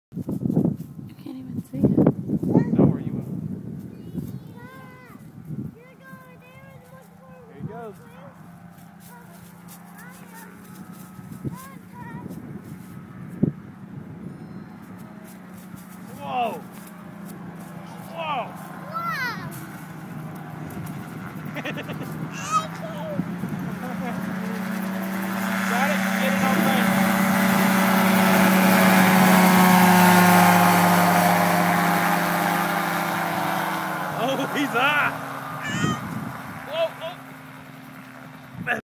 Gyrobee Gyrocopter Kennys first lift off (WOW)
Kenny doing the first lift off in this nice Gyrobee gyrocopter